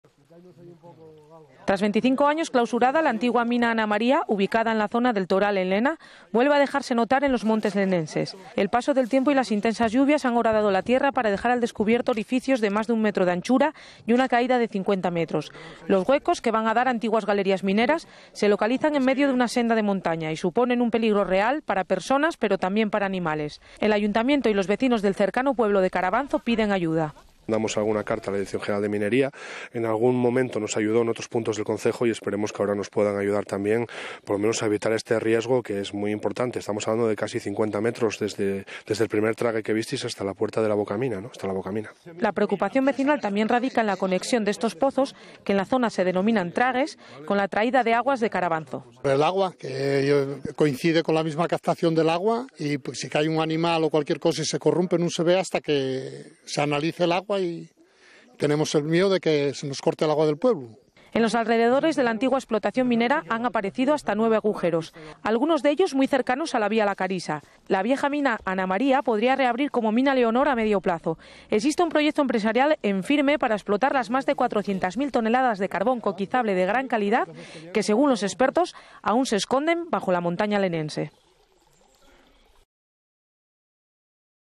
RTPA: Una antigua mina de Lena, preocupa seriamente a los vecinos de Carabanzo
Tras 25 años clausurada la antigua Mina Ana María, ubicada en la zona de el Toral en Lena, vuelve a dejarse ver y notar en los montes leneses.

El paso del tiempo y las intensas lluvias han horadado la tierra para dejar al descubierto orificios de más de un metro de anchura y una caída de 50 metros.

Los huecos, que van a dar a antiguas galerías mineras, se localizan en medio de una senda de montaña y suponen un peligro real para personas pero también para los animales.

La preocupación vecinal también radica en la conexión de estos pozos, que en la zona denominan "tragues", con la traída de aguas de Carbanzo.

En los alrededores de la antigua explotación minera han aparecido hasta 9 agujeros, algunos de ellos muy cercanos a la vía La Carisa.

La vieja mina Ana María podría reabrir como Mina Leonor a medio plazo, ya que existe un proyecto empresarial en firme para explotar las más de 400.000 toneladas de carbón coquizable de gran calidad que, según los estudios, aún se esconden bajo la montaña lenense.